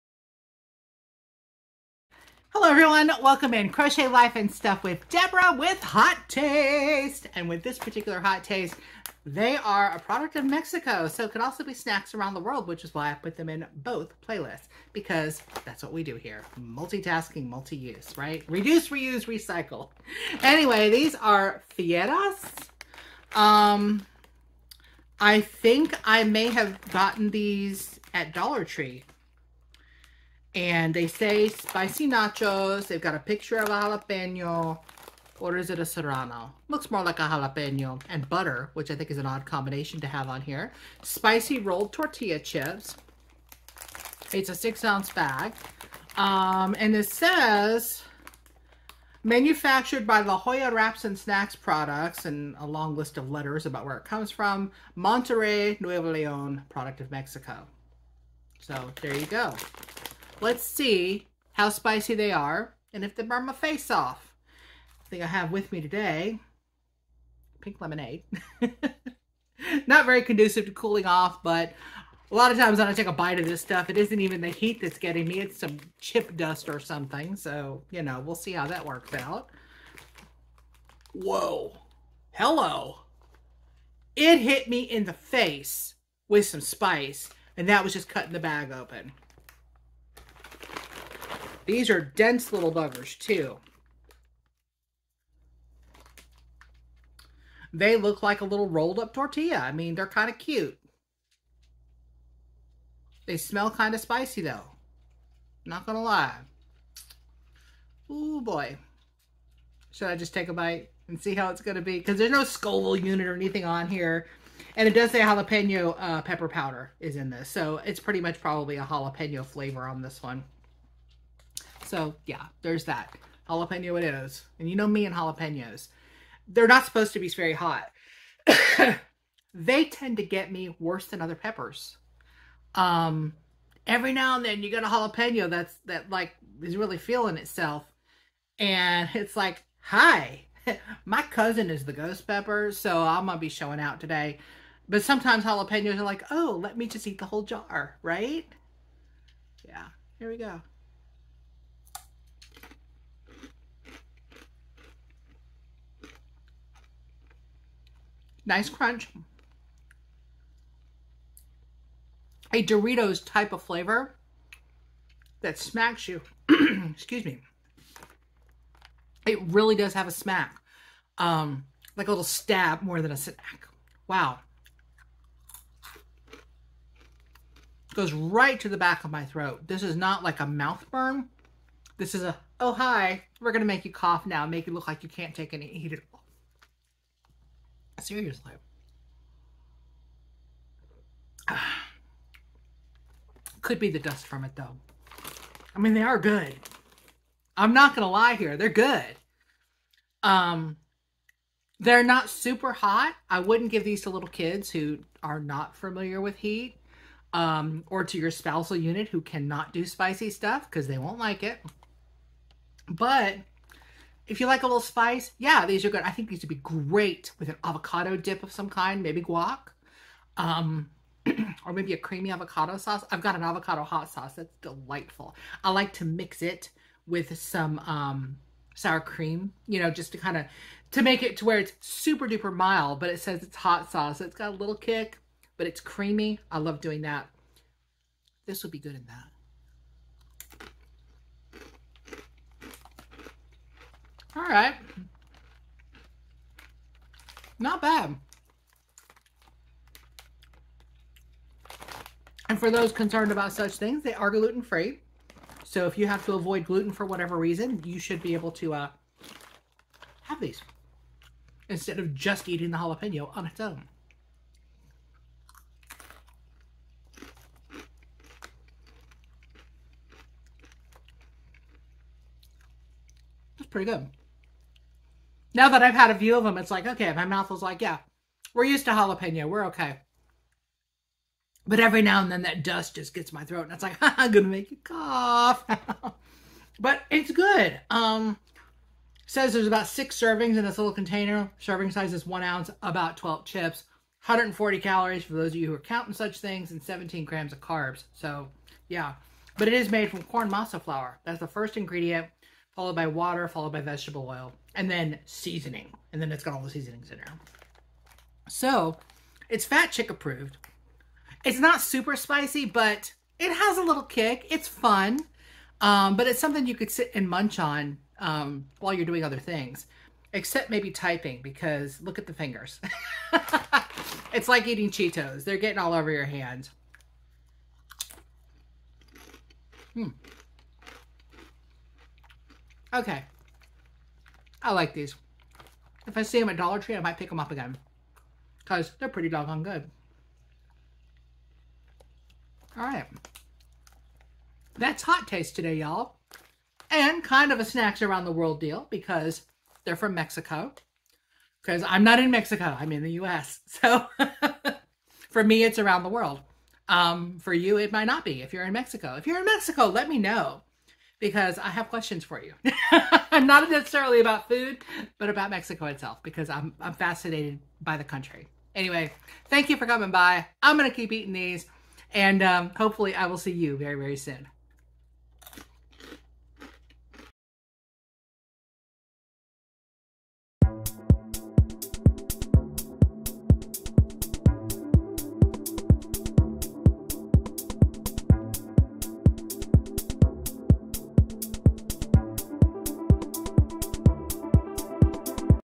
Hot Taste! Fieras-Spiy Nachos - How Spicy are they?- Crochet, Life, & Stuff with Debra
Yay snacks - are they FatChickApproved?

I purchase the subscription to @munchaddict with my own money. Not sponsored. Sadly.